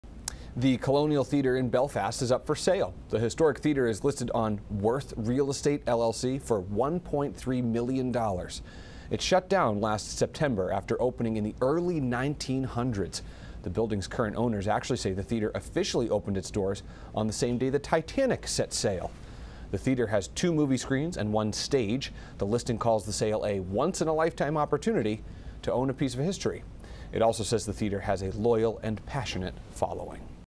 Historic movie theatre in Belfast is up for sale
The sale of Belfast's Colonial Theatre serves as a unique chance to own a piece of Maine history.